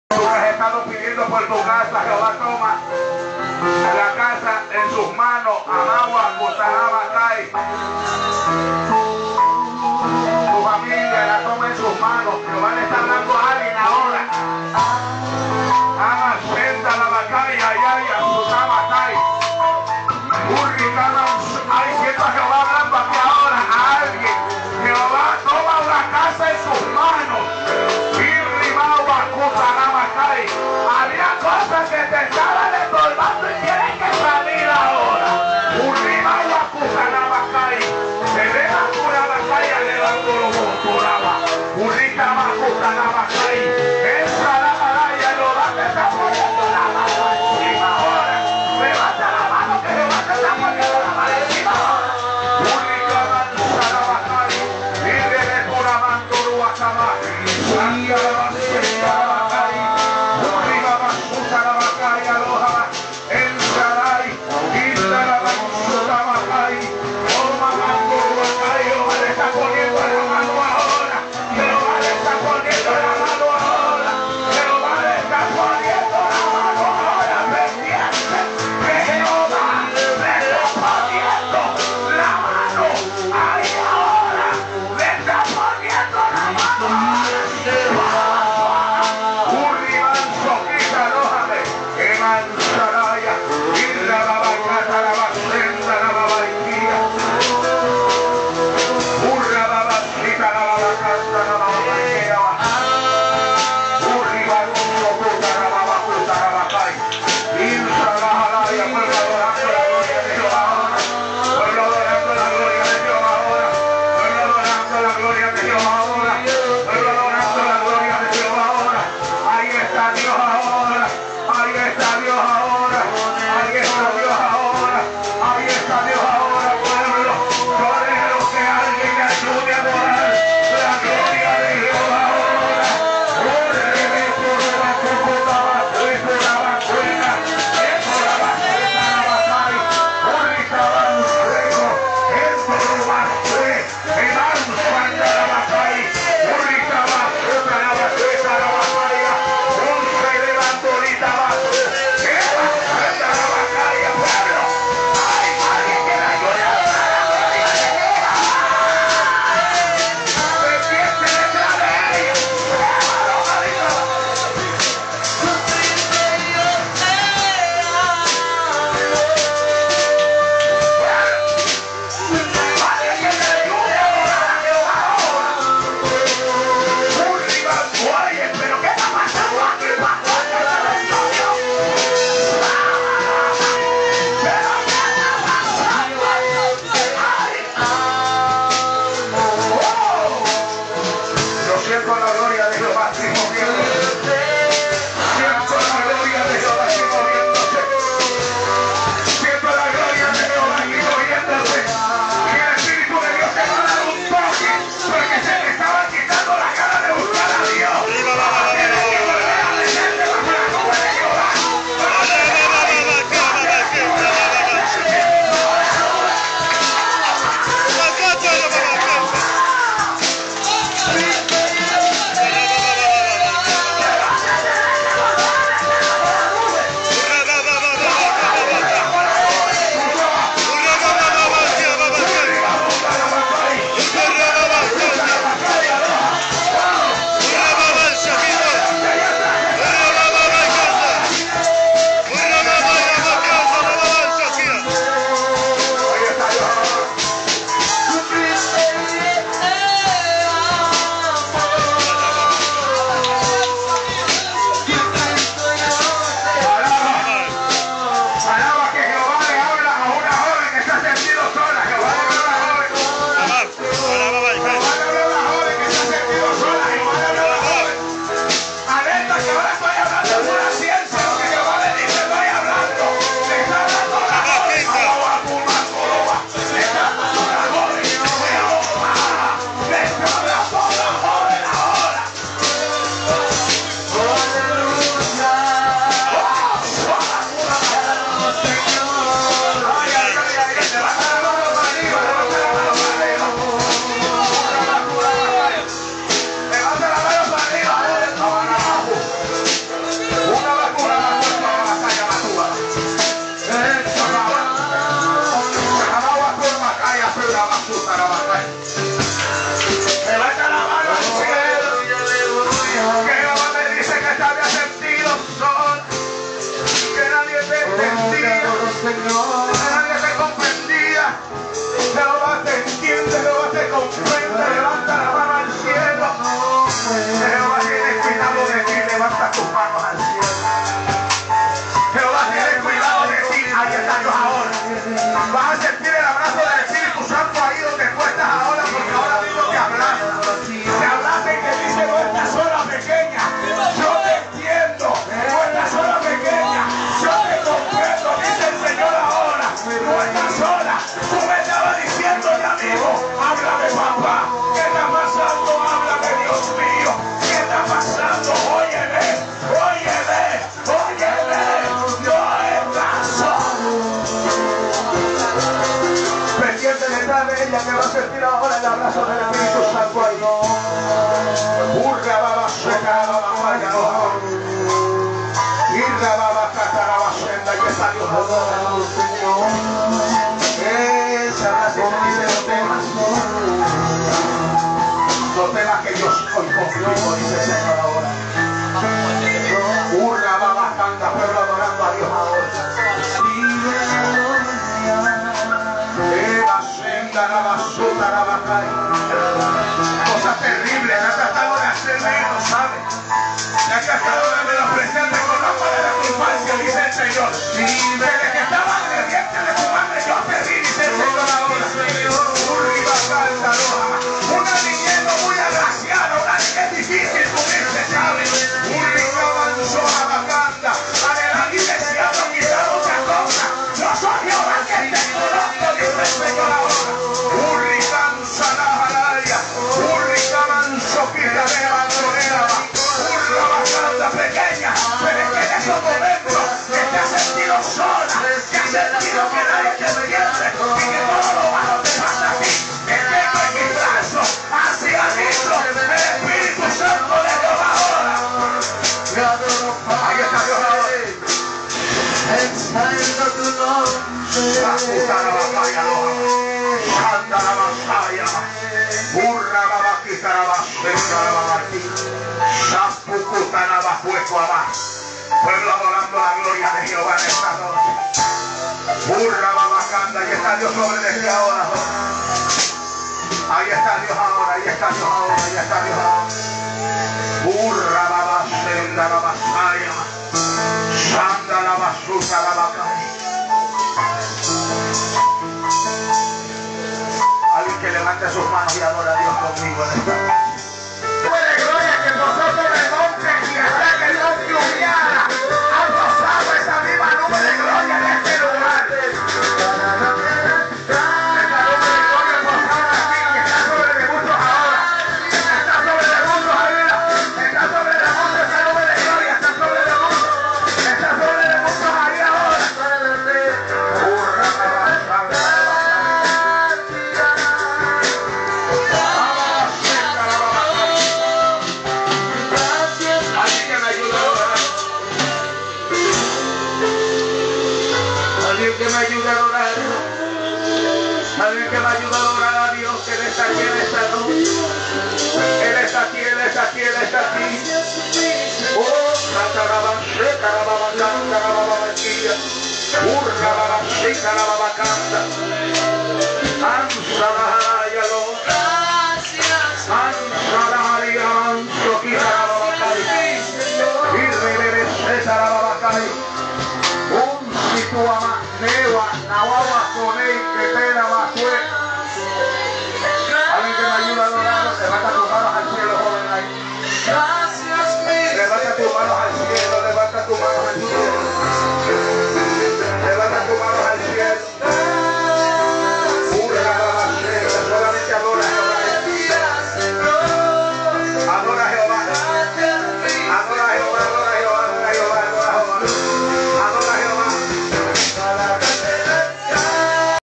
Evangelista orèste gutiérrez..MP4
DIOS ministrando las vidas
saludos a todos los hermanos en cristo quienes estan orando por este ministerio.
esperamos q sean de bendicion estos vodeos candela¡¡¡¡¡¡¡
seguimos adelante